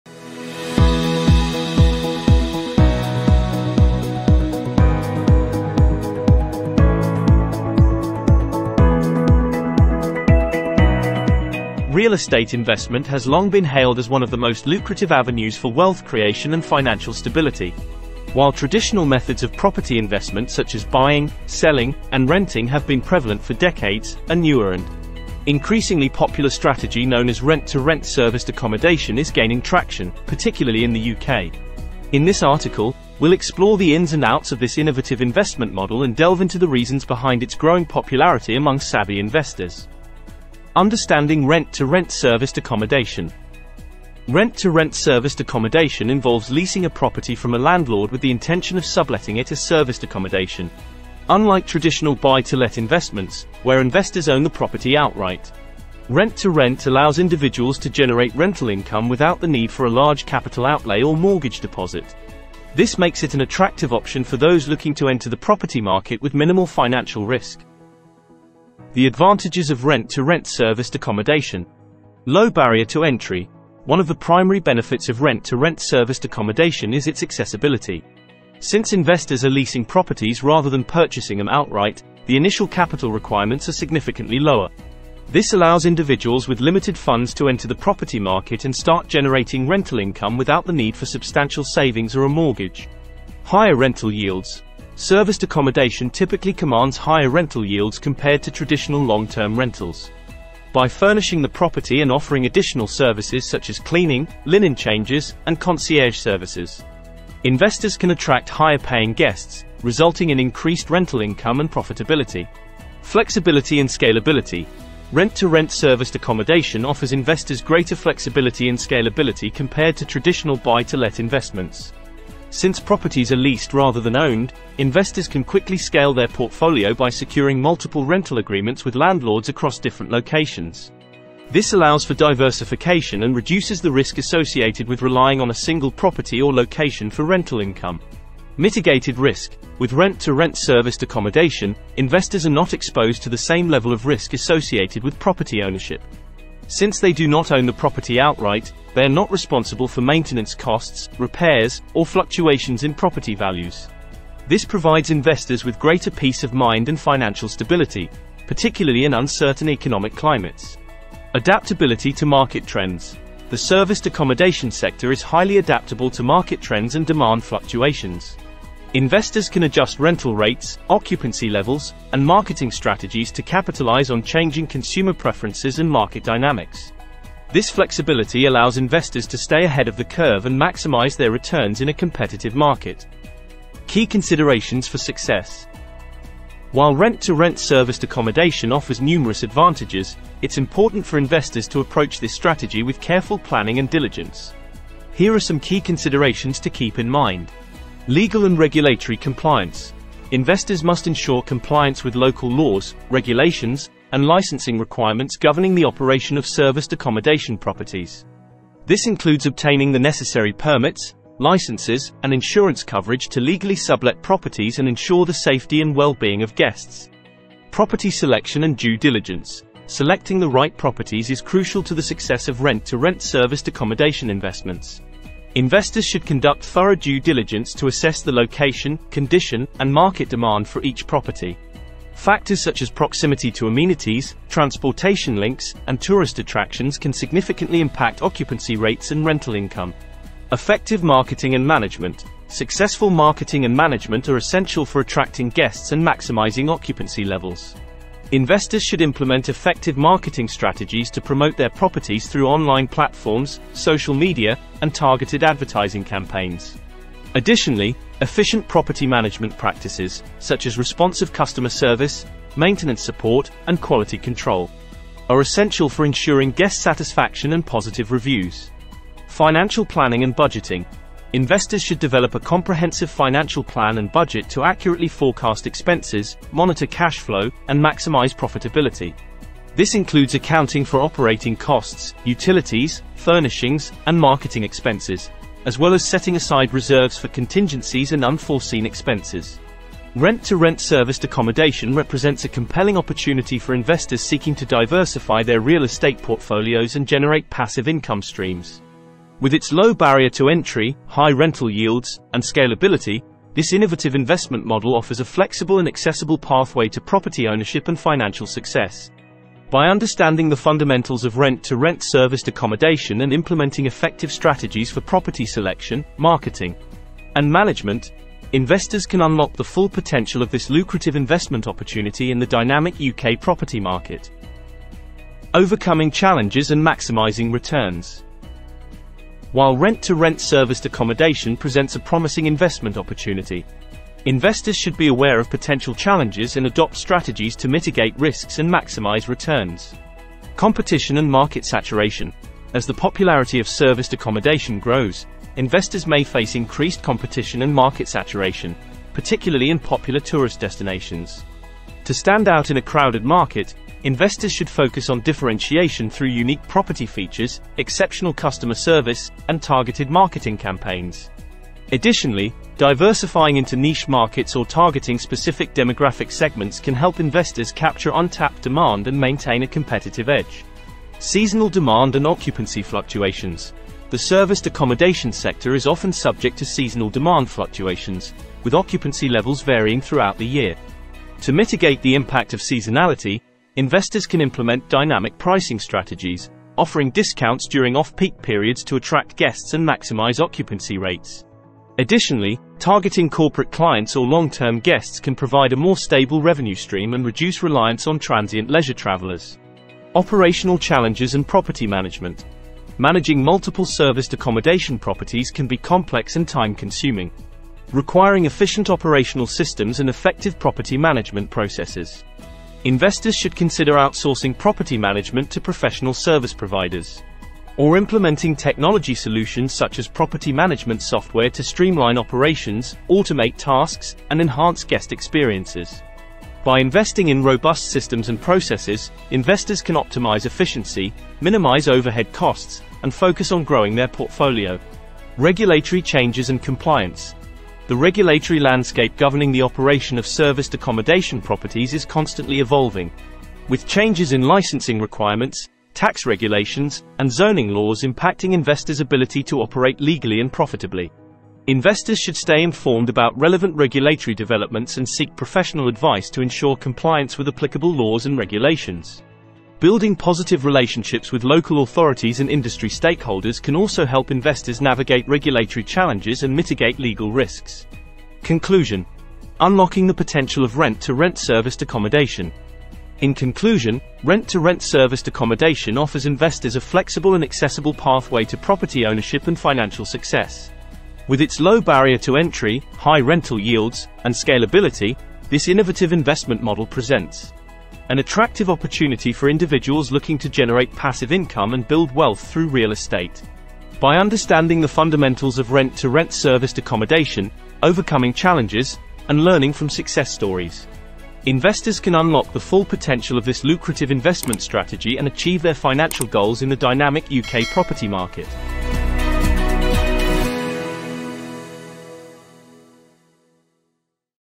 World of Rent-to-Rent Serviced Accommodation in the UK - Power of Real Estate Investments
Unlock the power of real estate investments with our exploration of the world of rent-to-rent serviced accommodation in the UK!
Discover the lucrative opportunities available in the dynamic UK property market and learn how to leverage rent-to-rent strategies for maximum returns. From understanding the fundamentals of real estate investing to implementing effective rental property management techniques, this video provides valuable insights and expert advice for aspiring investors. Whether you're a seasoned investor or a beginner, our comprehensive guide equips you with the knowledge and tools to succeed in the competitive world of real estate investment.
Don't miss out on this opportunity to tap into the power of real estate and build wealth through rent-to-rent serviced accommodation in the UK!